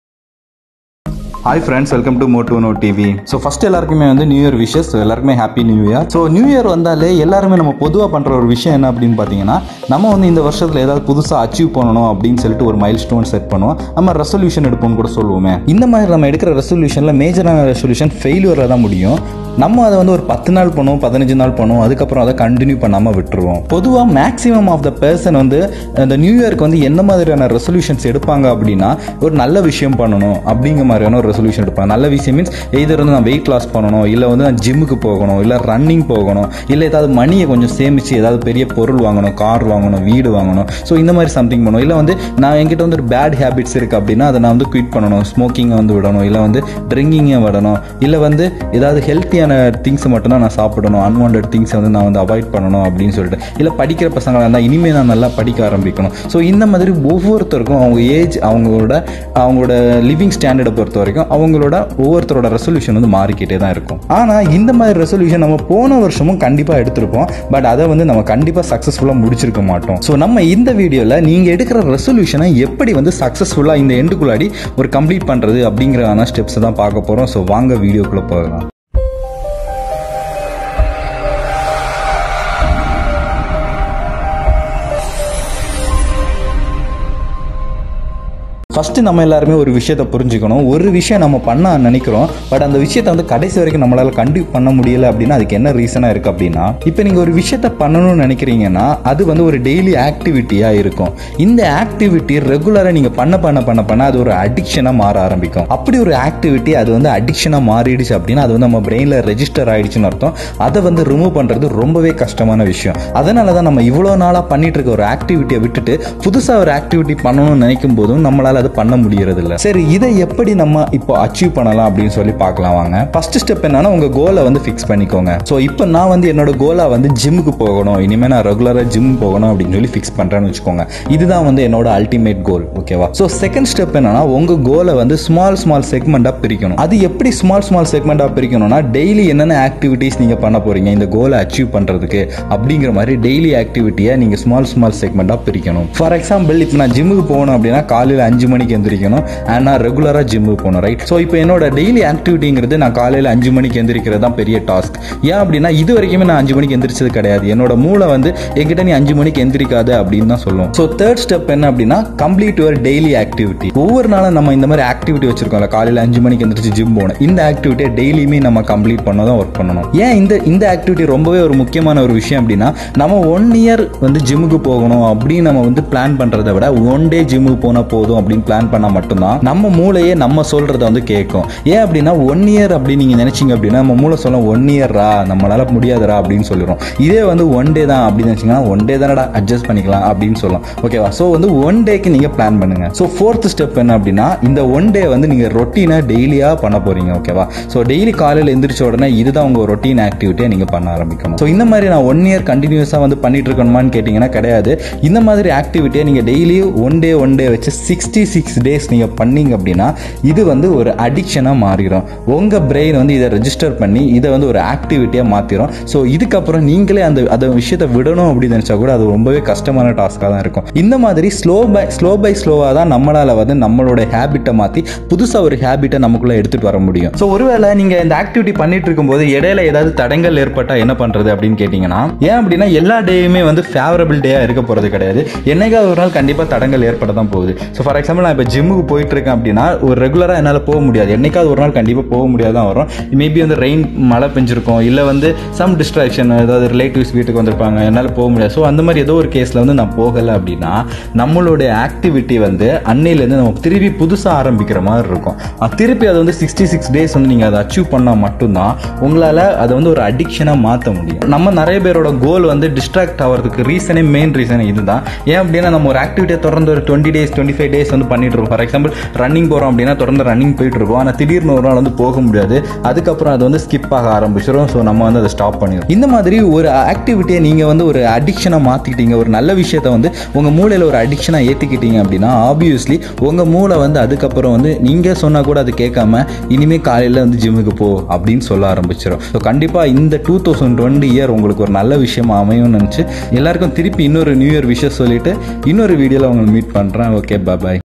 2020ல் உங்க வாழ்கையில் மாற்றம் கொண்டு வருவது எப்படி? | How to make your Newyear Resolution Success?
This video explain about how to make your newyear resolution a grand success.